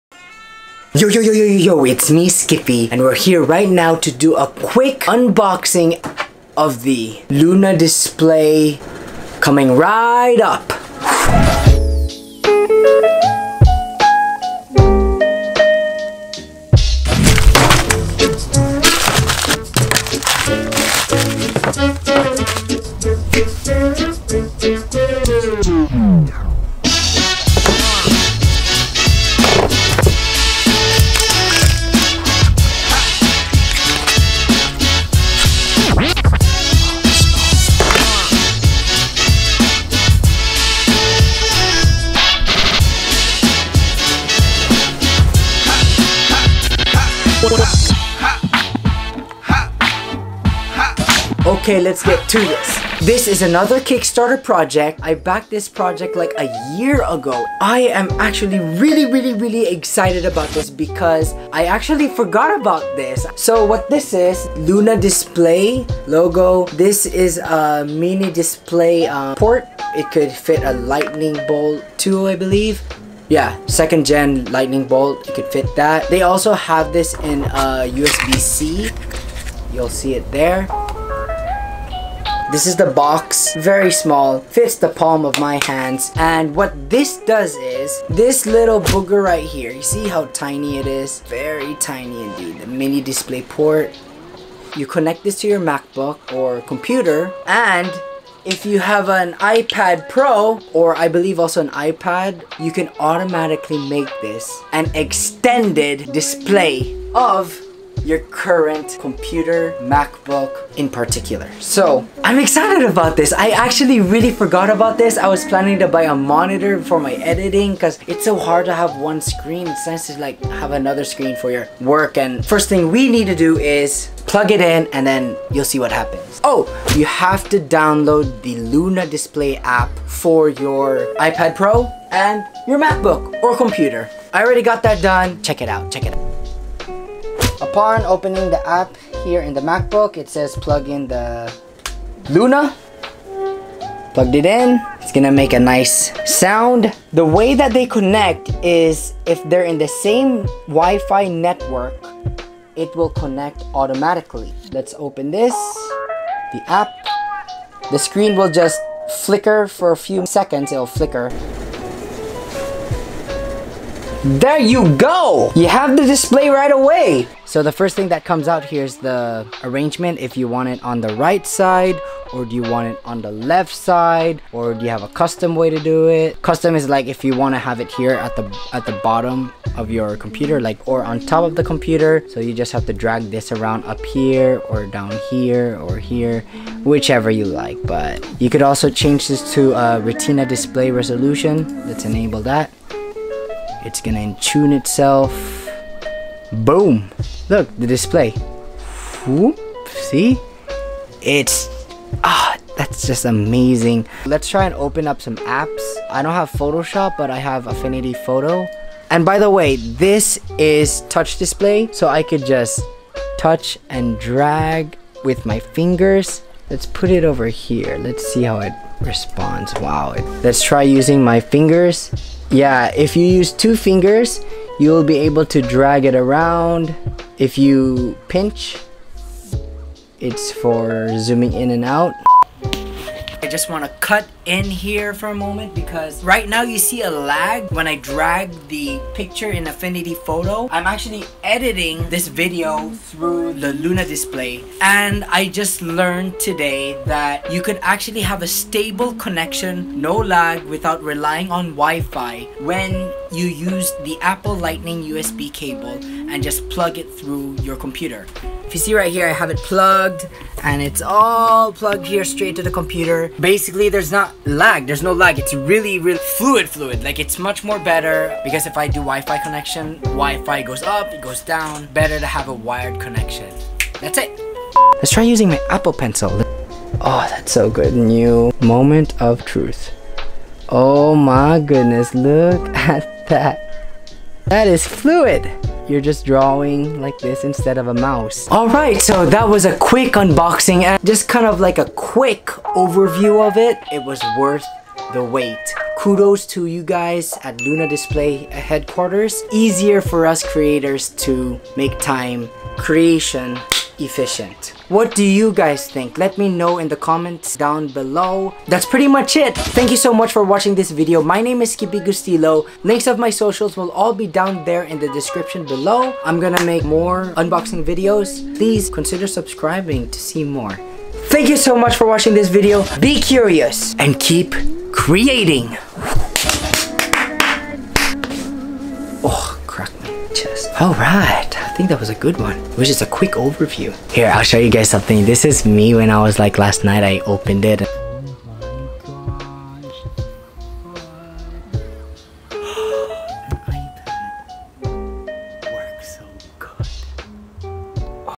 Unboxing the LUNA DISPLAY | A Kickstarter Project
Just got this Kickstarter project the LUNA DISPLAY by Astro HQ. Decided to do an Unboxing & quick overview of it. This product turns your iPad/iPad Pro into an Extended second display! How Amazing is that! Took a year of waiting but, IT WAS WORTH IT!